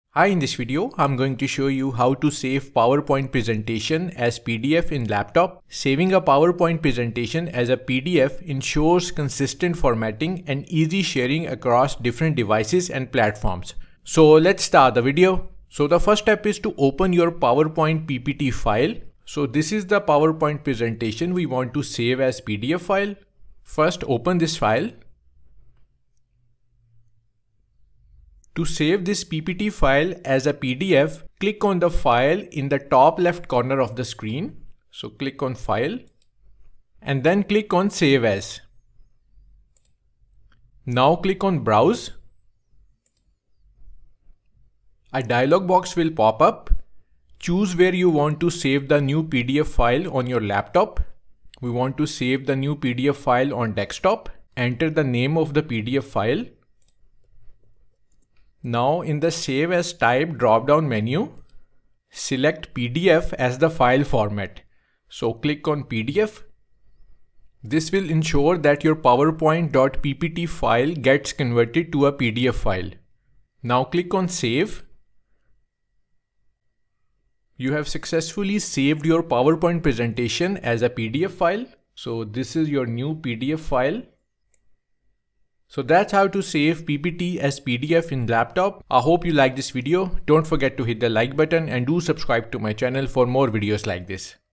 How to Save PowerPoint as PDF
This video guides about how to save powerpoint as pdf in laptop.

To learn how how to save ppt as pdf, follow the step-by-step guide.

Saving a PowerPoint presentation as a PDF ensures consistent formatting and easy sharing across different devices and platforms.

Here are the Steps to Save a PowerPoint Presentation as a PDF File:

Step 1: Open your PowerPoint Slides.
Step 2: Click on "File" and then select "Save As".
Step 3: Choose a filename for the new PDF.
Step 4: In the "Save as type" dropdown menu select the "PDF" format .
Step 5: Click on Save button to complete the conversion.

Your PowerPoint slides will be converted and saved as a pdf file.

So that's how to save powerpoint as pdf. If you still have trouble saving PPT file as pdf in laptop , ask in the comments section below, and I'll surely reply.